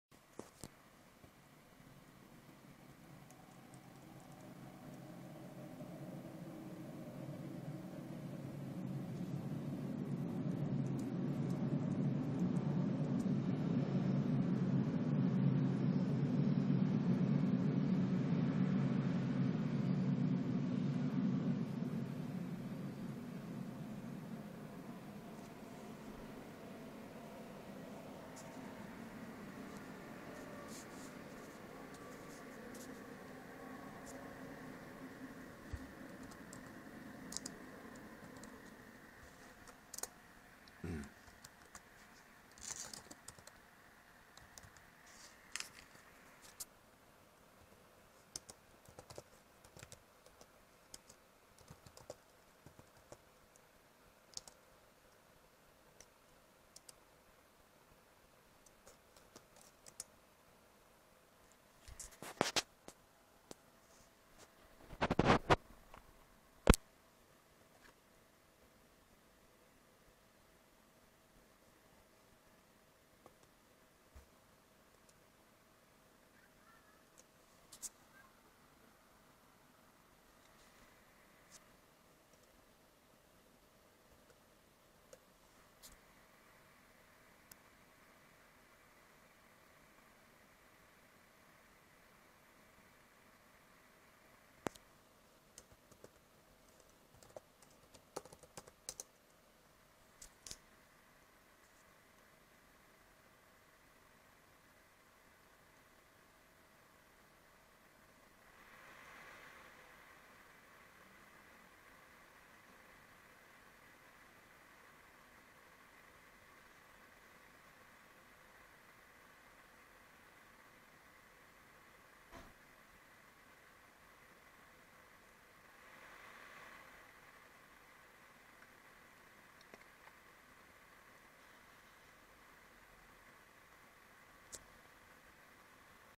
st tweak grub2 theme change
...at runlevel 1, as i figured that my grub2 boot menu wasnt displayed, so i could use "Advanced Options" to boot into "Emergency Stage".
Therefor introducing my grub2 theme nasa circle hubble galaxy)